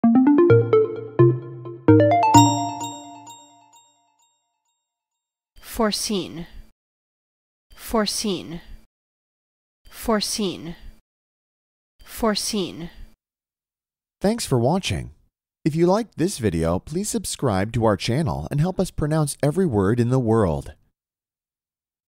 How To Pronounce Foreseen - Pronunciation Academy
Learn how to pronounce Foreseen

This is the *English* pronunciation of the word Foreseen.